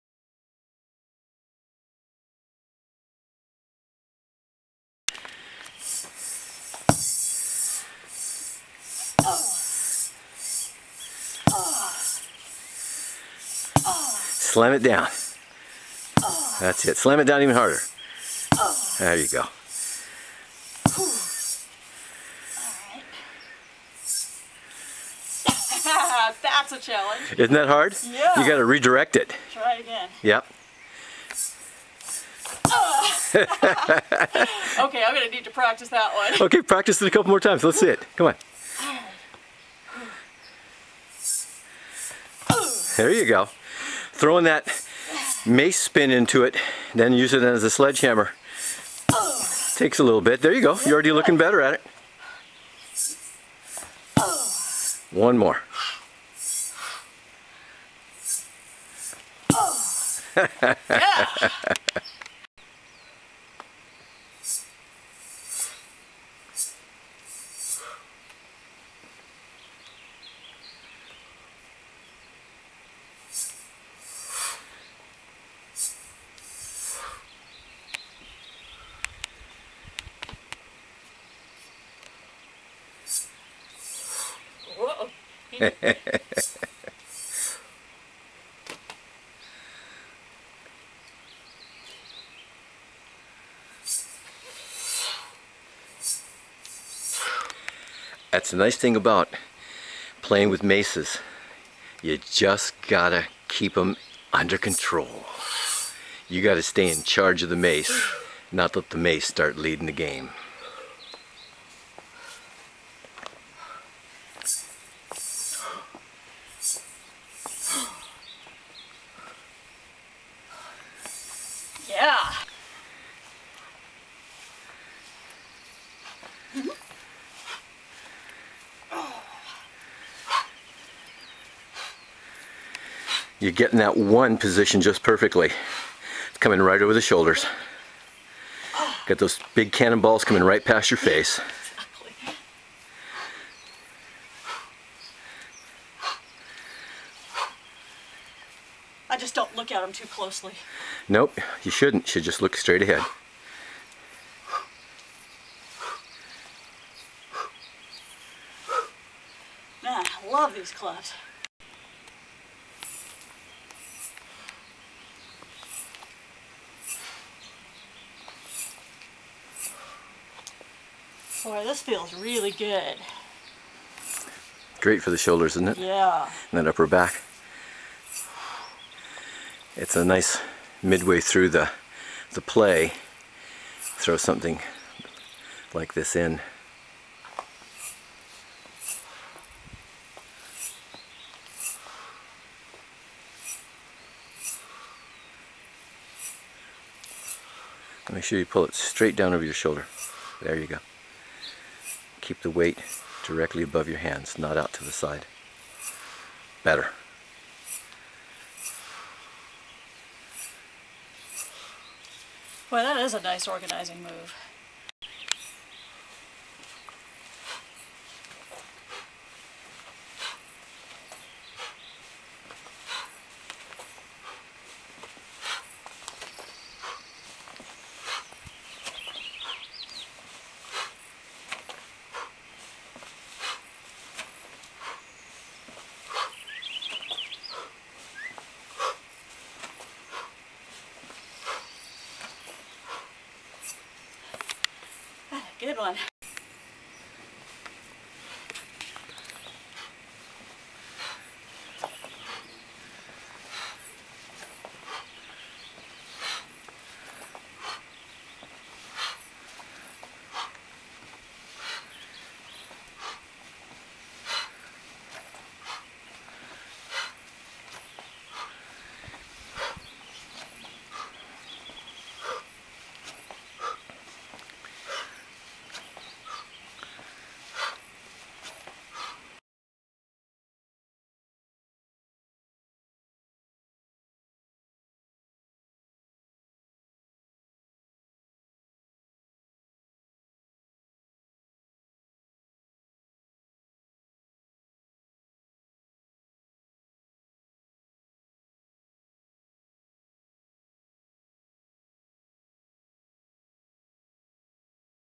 StrongerGrip Workout
We did a lot of different moves using ONLY gear that came from Ryan at StrongerGrip.com. Love his gear-love the possibilities.